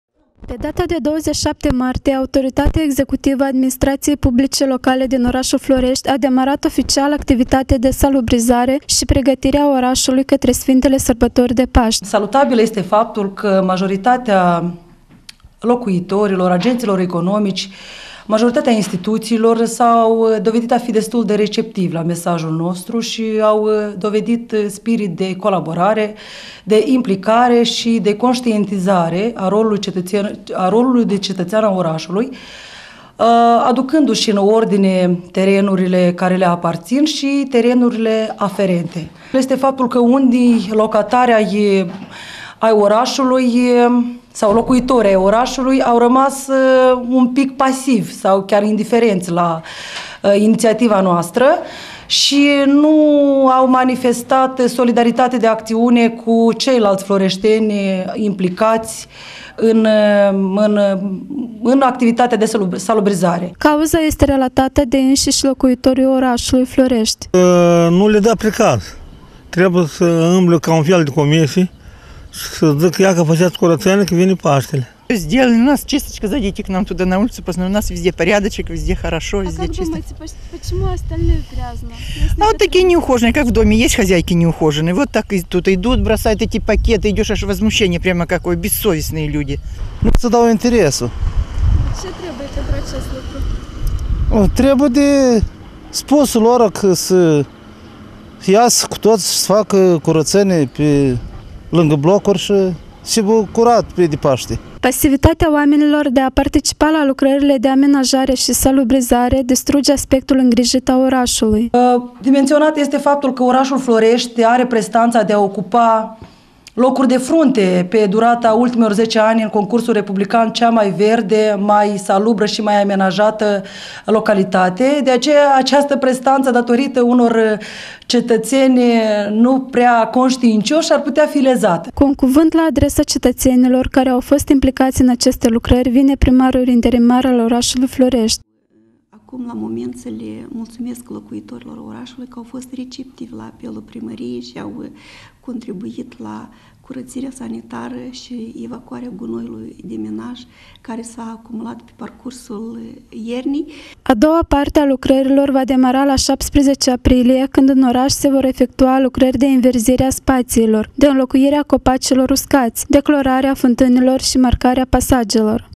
TOTALIZĂRI A LUCRĂRILOR DE SULUBRIZARE A or FLOREŞTI
PE DATA DE 27 MARTIE, AUTORITATEA EXECUTIVA A ADMINISTRATIEI  PUBLICE LOCALE DIN ORASUL FLORESTI , A DEMARAT OFICIAL   ACTIVITATEA  DE  SALUBRIZARE  SI PREGATIRE  A   ORASULUI CATRE  SFINTELE STBATORI DE PASTI. CAUZA ESTE RELATATA  DE INSISI  LOCUITORII  ORASULUI  FLORESTI. PASIVITATEA OAMENILOR   DE  A PARTICIPA  LA LUCRARILE DE AMENAJARE SI SALUBRIZARE, DISTRUGE ASPECTUL INGRIJIT  A ORASULUI. CU UN CUVINT LA ADRESA CETATENILOR CARE AU FOST IMPLICATI IN ACESTE LUCRARI, VINE PRIMARUL- INTERIMAR AL  ORASULUI FLORESTI. A DOUA PARTE A LUVRARILOR VA DEMARA LA 17 APRILIE, CIND IN ORAS SE VOR EFECTUA  LUCRARI DE INVERZIRE A SPATIILOR, DE INLOCUIRE  A  COPACILOR USCATI, DE CLORARE A FINTINILOR SI MARCAREA PASAJELOR.